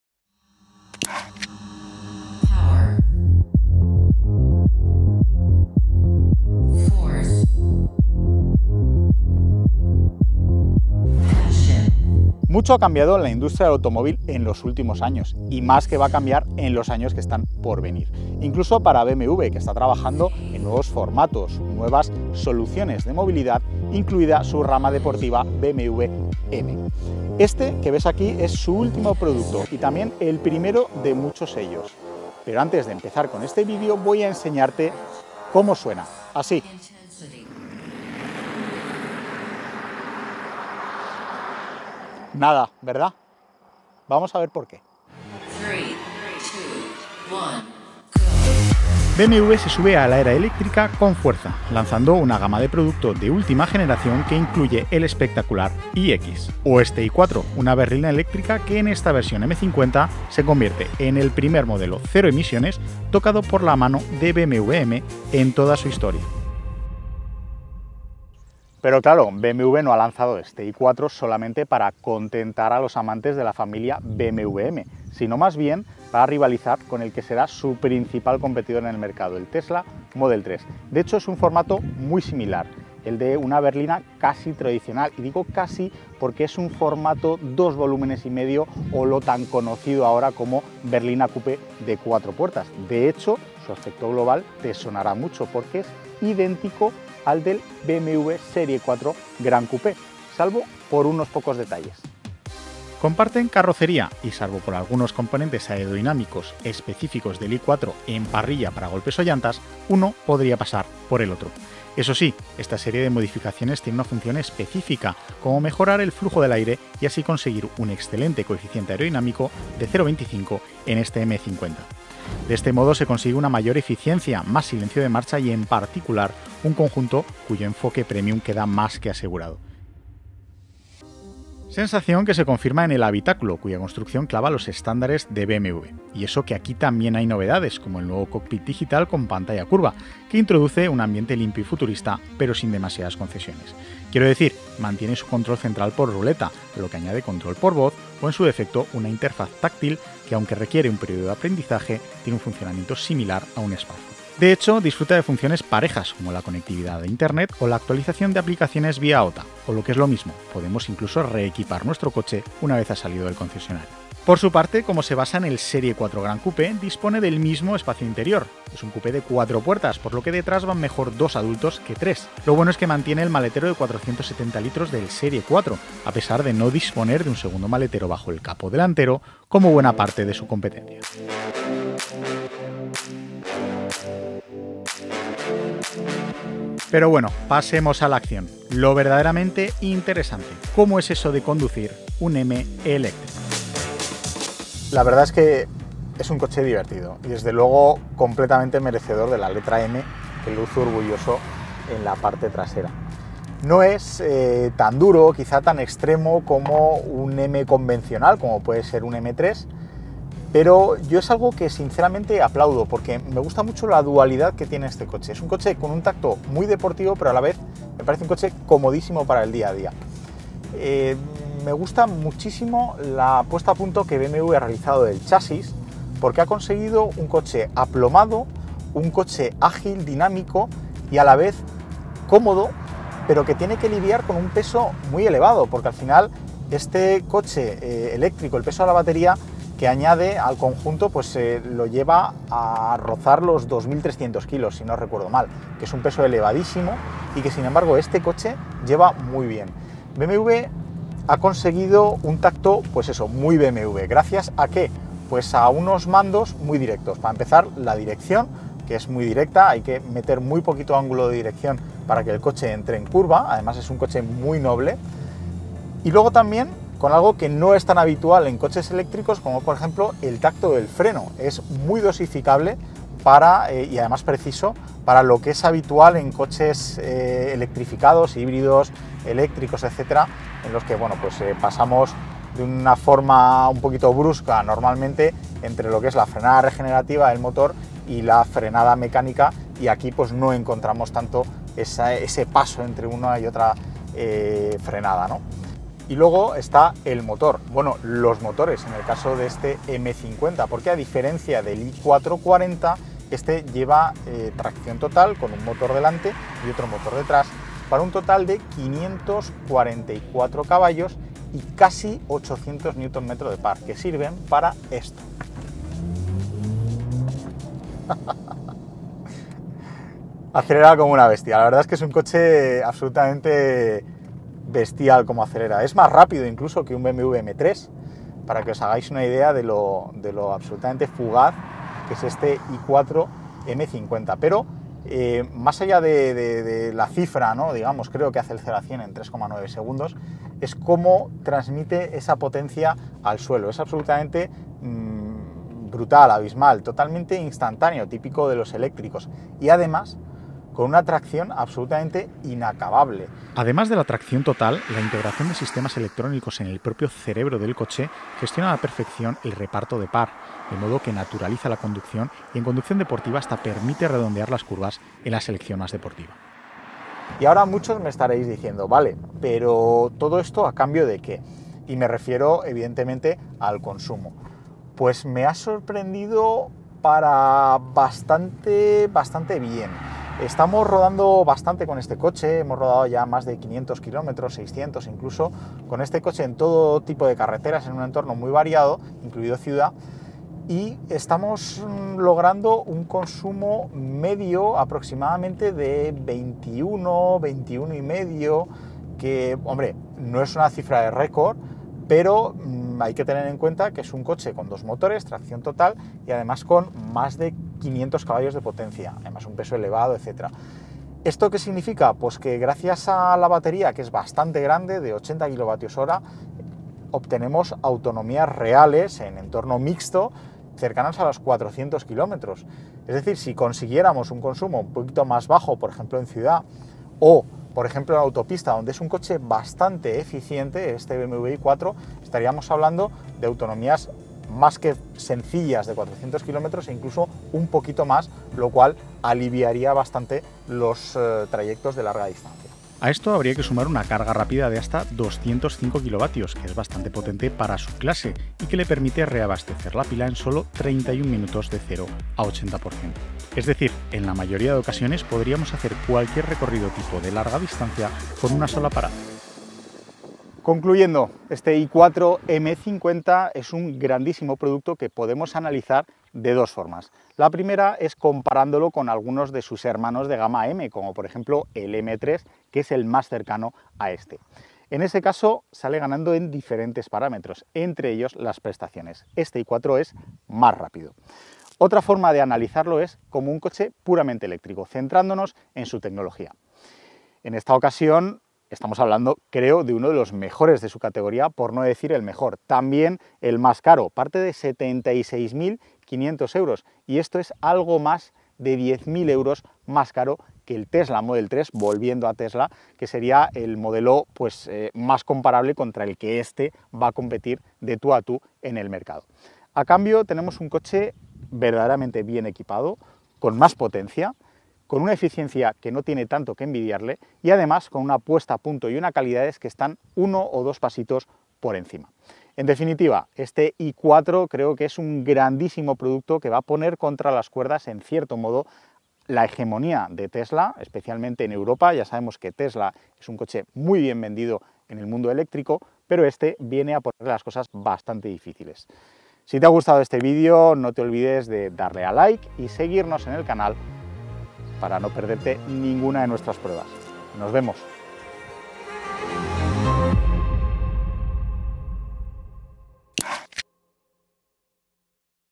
BMW i4 M50 2022 | Eléctrico | Prueba a fondo - Clicacoches.com
Ponemos a prueba el nuevo BMW i4, una berlina 100% eléctrica llegada para plantar cara a Tesla. Lo hacemos en su variante M50, la más potente por el momento y, de paso, la primera incursión del brazo deportivo de BMW en la tecnología eléctrica.
Se trata de un coche premium, de muy alta calidad y con una tecnología encabezada por su cockpit digital heredado del tope de gama iX.
Mecánicamente incorpora una pareja de motores para ofrece tracción total y una cifra de 544 CV de potencia. Con esto en la mano es más rápido que todo un BMW M3... pero también más pesado. En cualquier caso BMW M ha trabajado extensamente en la puesta a punto del chasis para hacer de este i4 no solo un digno competidor en el mercado, sino para convertirlo en lo que claramente es la referencia de su segmento.